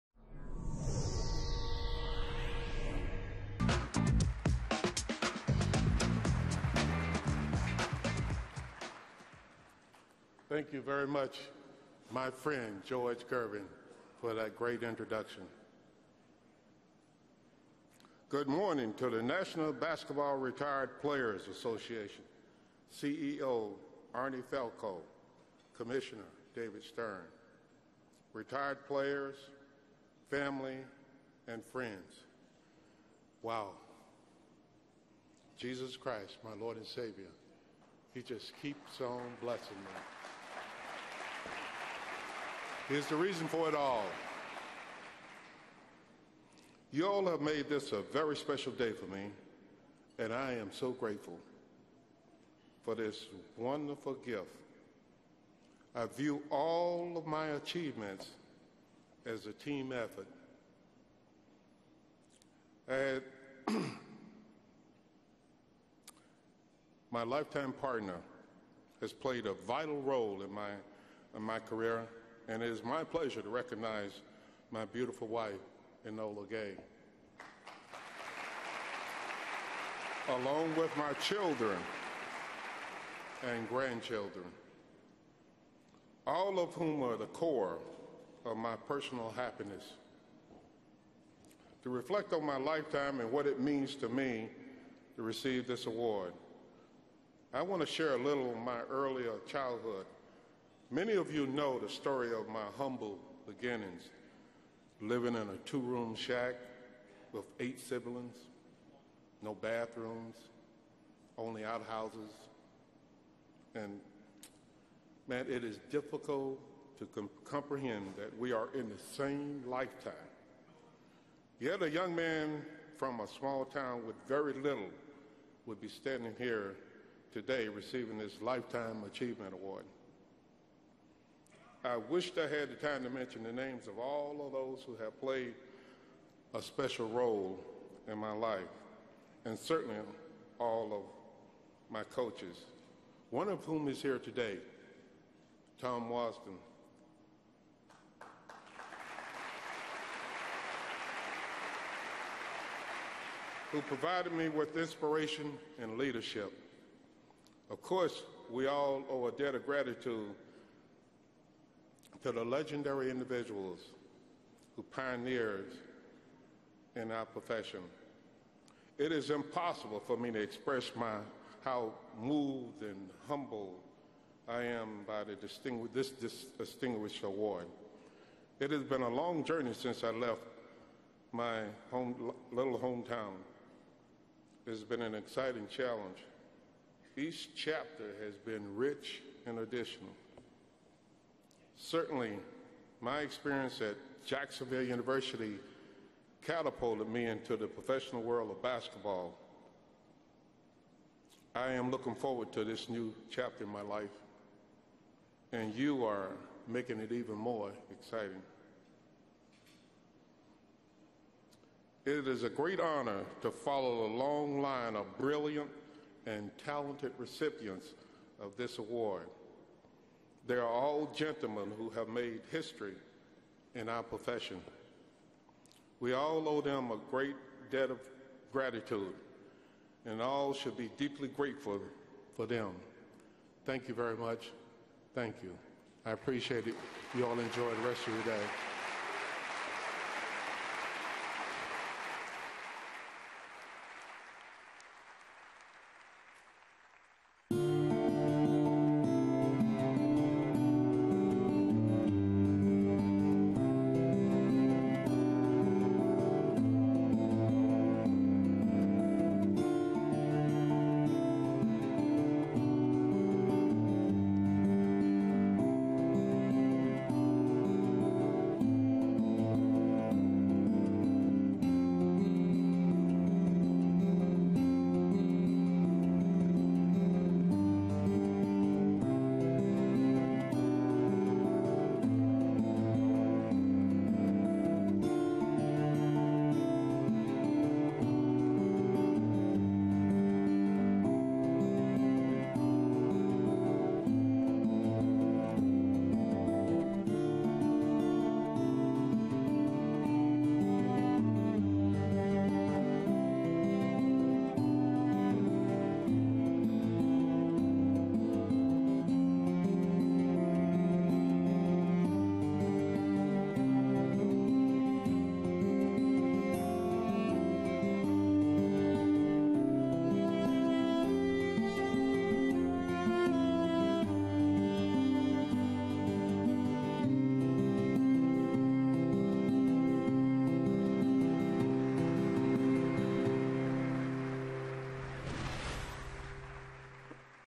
Legends Brunch: Memoriam
The Legends Brunch mourns the passing of their fallen NBA family members.